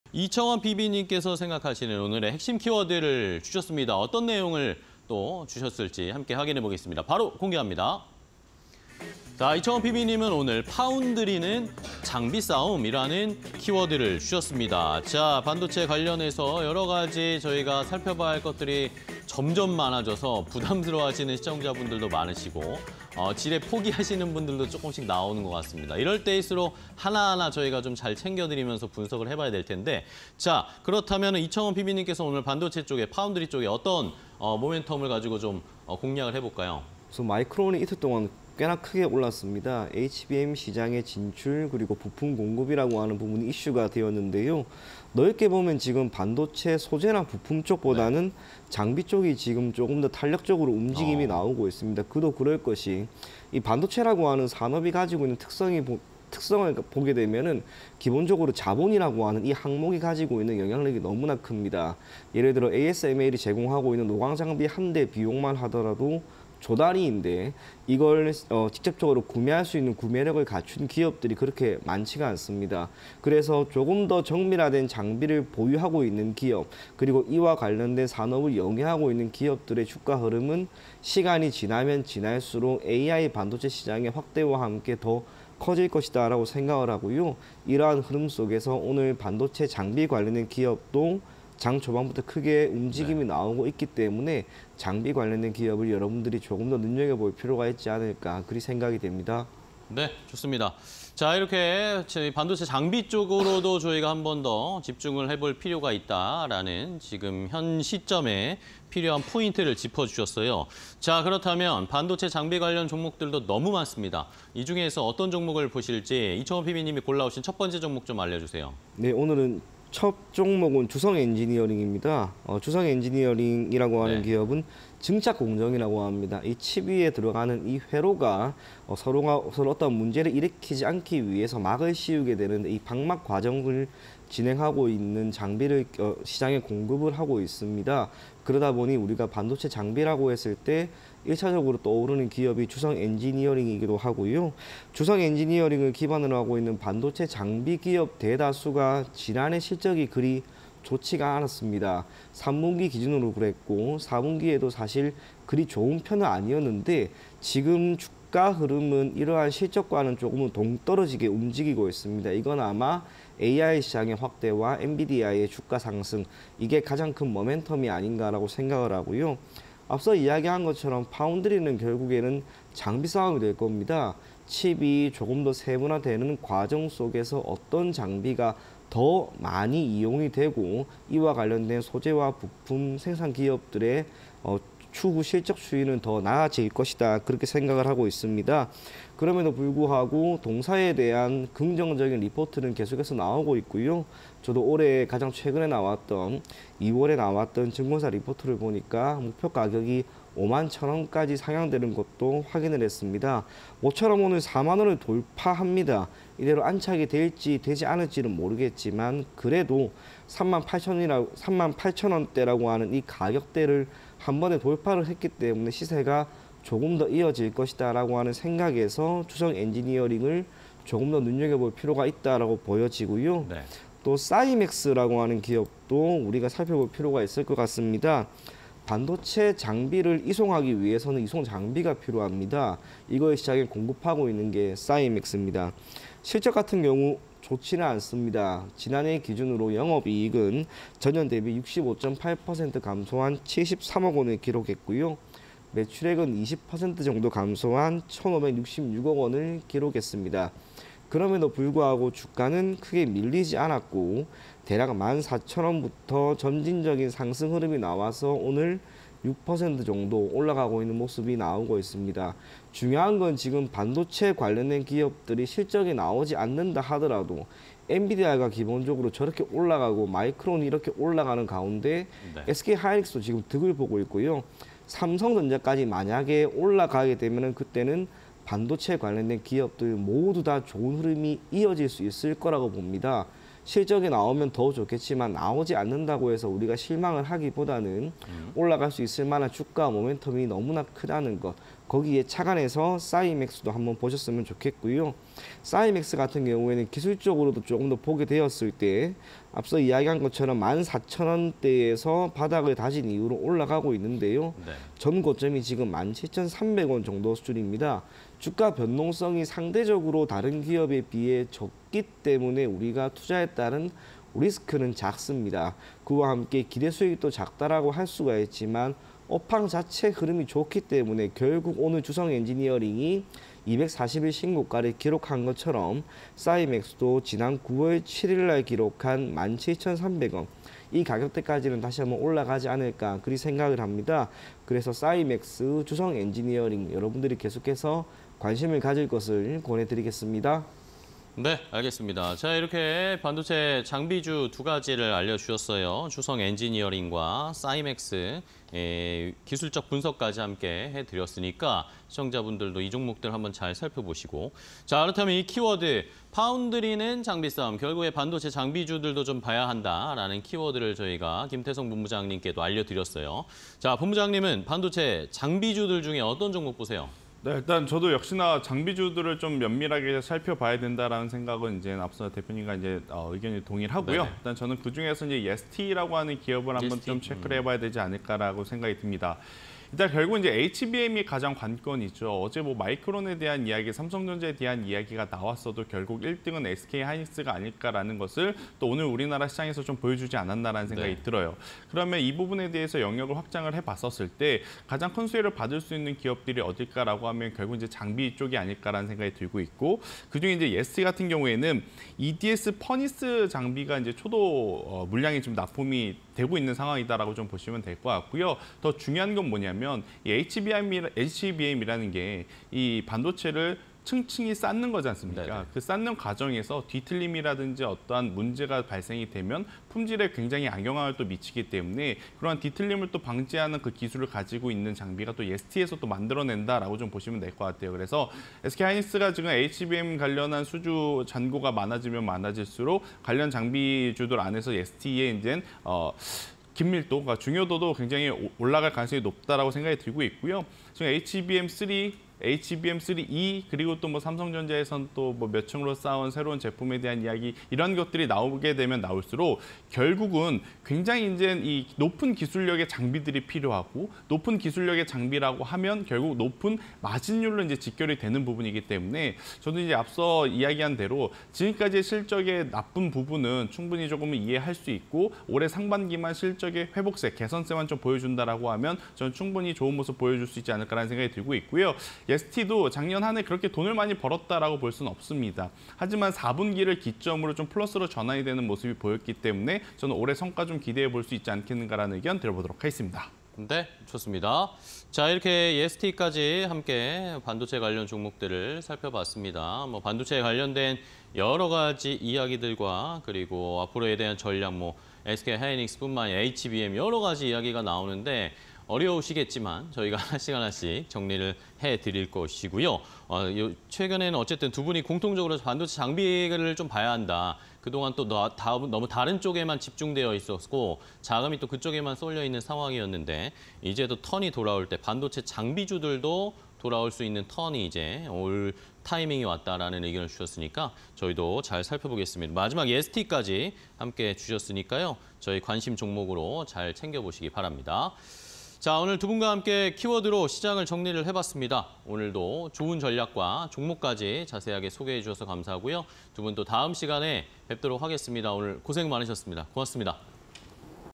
주성엔지니어링(036930), 싸이맥스(160980), 예스티(122640)... 반도체 장비_키워드 배틀 (20240228)
- 마이크론 2거래일 주가 상승
- 반도체 장비 정밀화 작업 집중
- 주성엔지니어링 등 관련주 상승세

주성엔지니어링(036930)
- 국내 반도체 제조장비 기업
- 5거래일 연속 상승... 52주 최고가 경신

싸이맥스(160980)
- 반도체 관련주 부각... 오늘장 상승 추세
- 실적 개선 기대감
- 로봇 테마 신성장 동력 기대

예스티(122640)
- 글로벌 기업향 HBM 핵심 장비 수주
- 고성능 HBM 장비 개발... 높은 마진율 확보